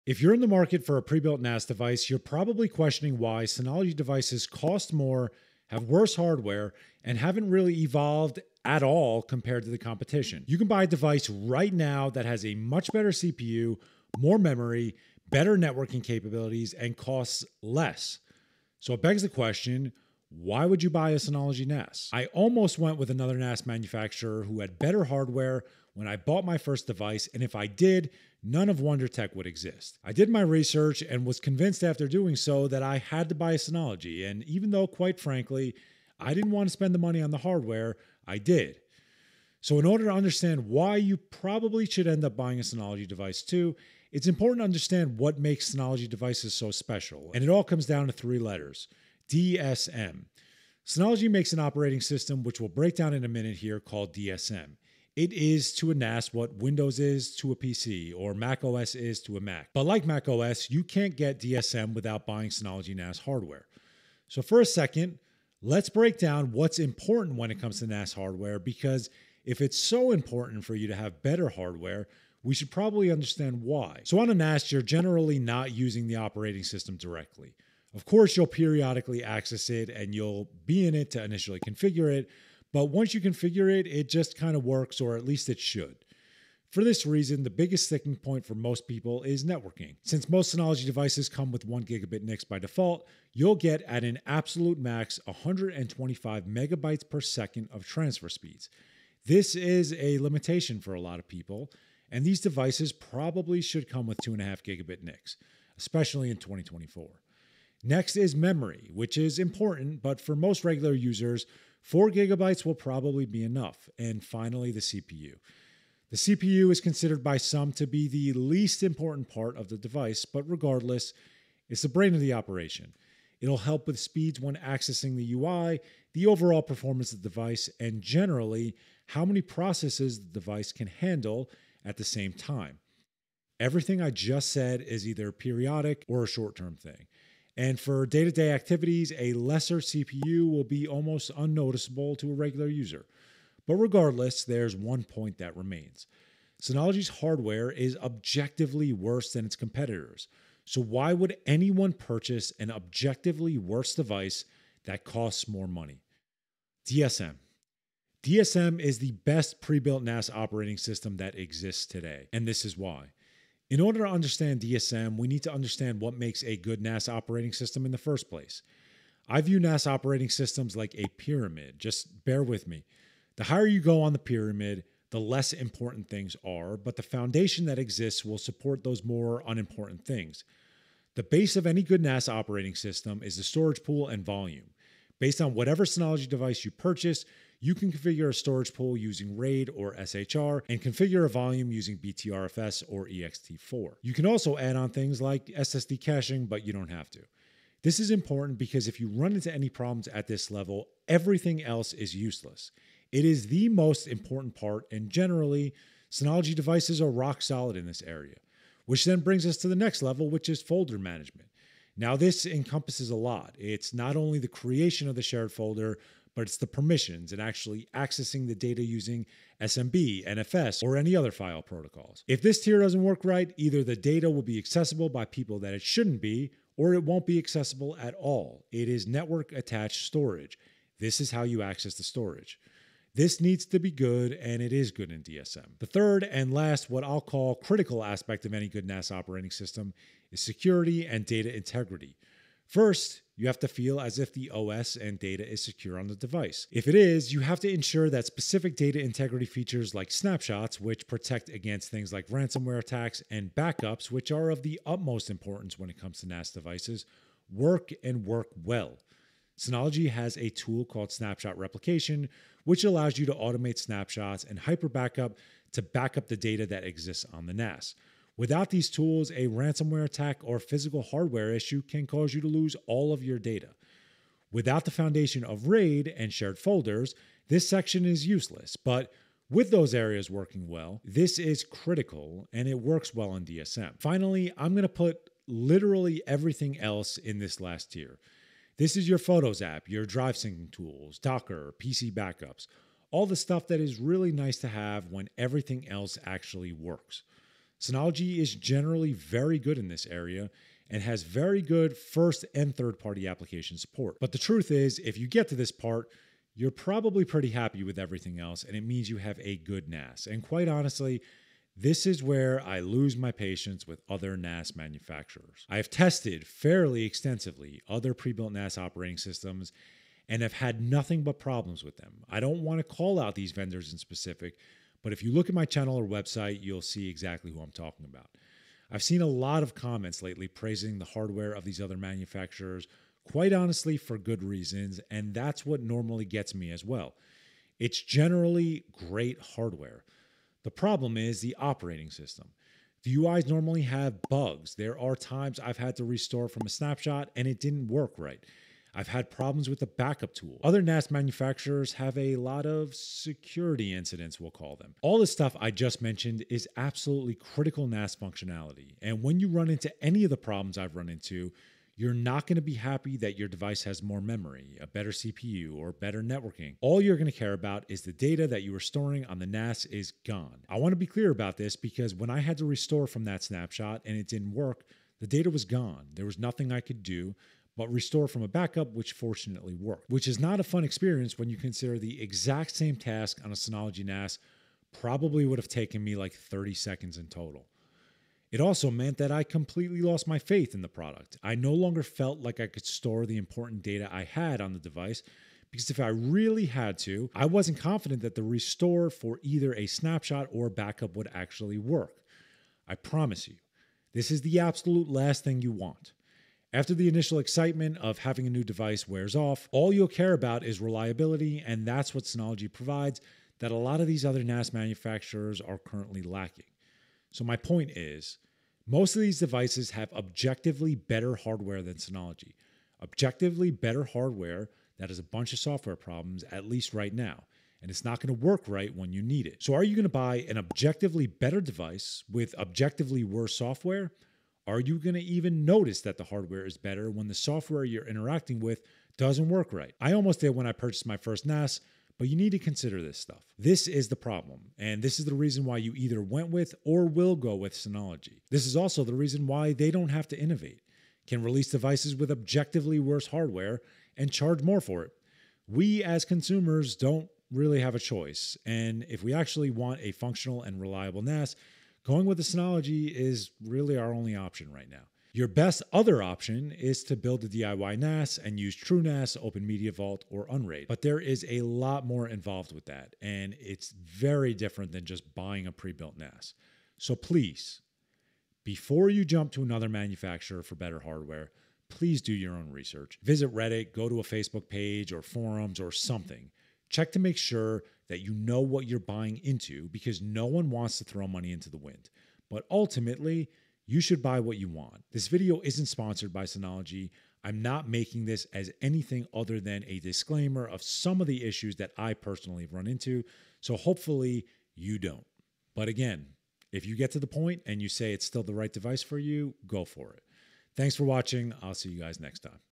Why would you EVER buy a Synology NAS?
Synology NAS devices have worse hardware and cost more than their competitors, so why would you ever buy one?

0:00 Intro
0:40 What Makes Synology Devices Good?
1:04 What's Important for NAS Hardware?
2:35 DiskStation Manager (DSM) and Why It's Good
5:10 What Other NAS Brands Get Wrong
7:20 Why You Bought a Synology NAS
7:56 DO YOUR OWN RESEARCH